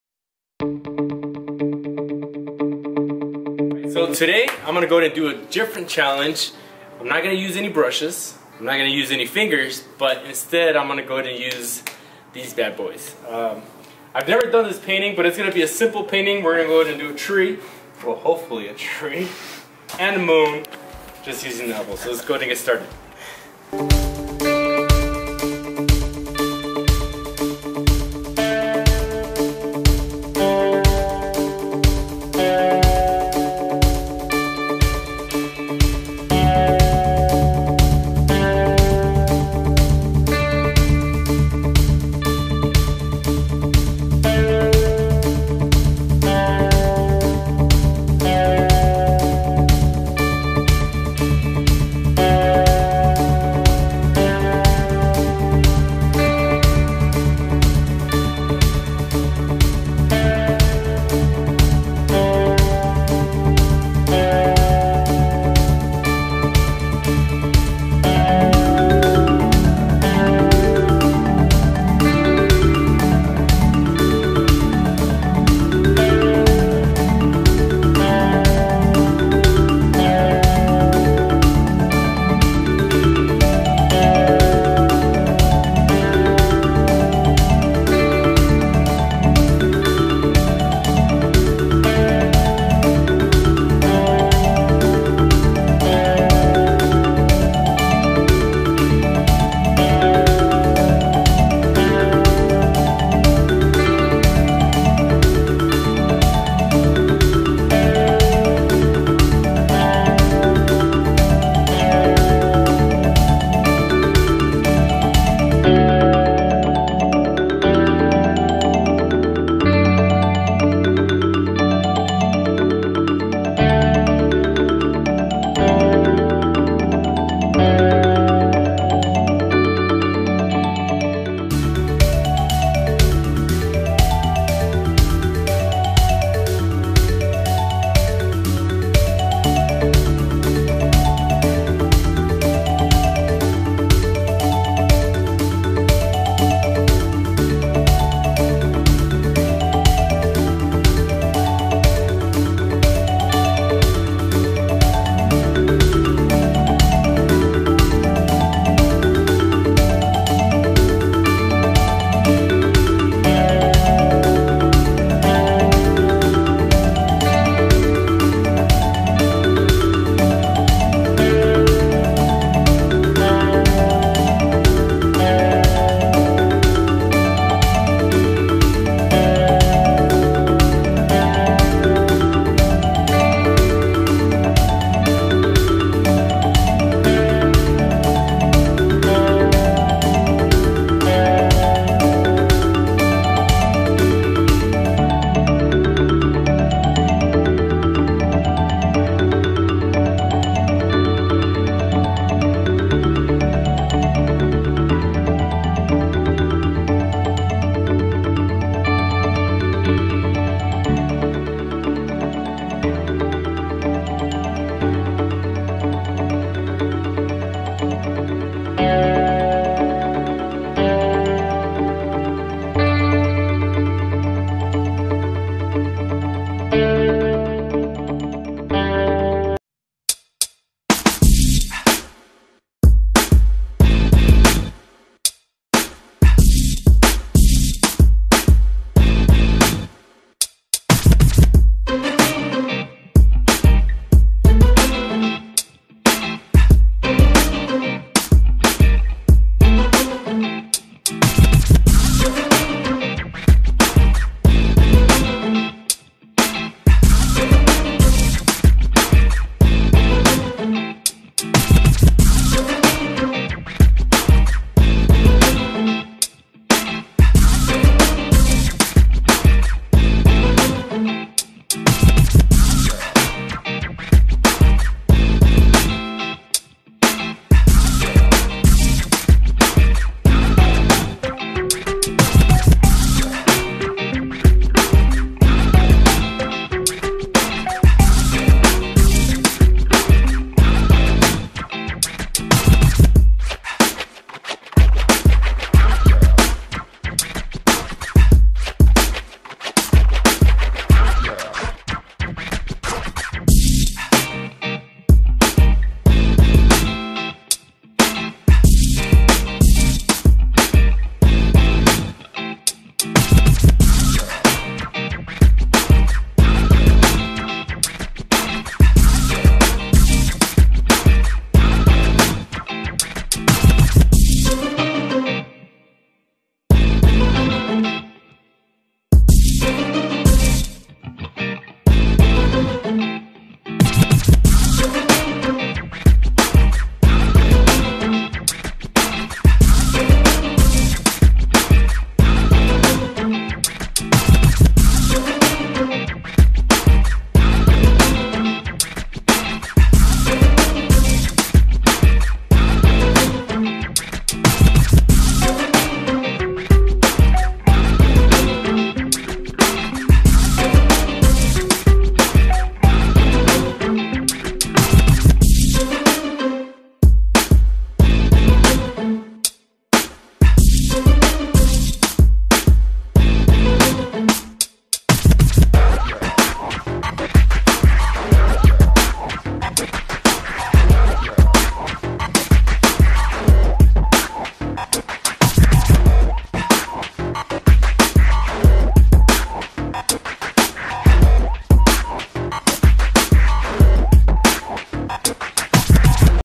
Painted with my Elbows...
i painted this beautiful painting using only elbows.

follow on ig  @memoscanvas
artist Guillermo Luna